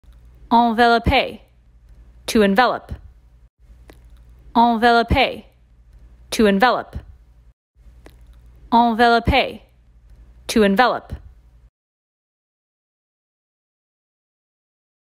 How to Pronounce Ballet Terms | Enveloppe
School of Ballet 5:8 presents an audio glossary of ballet terminology. Listen along and learn how to pronounce difficult french terms. For more information on School of Ballet 5:8 visit our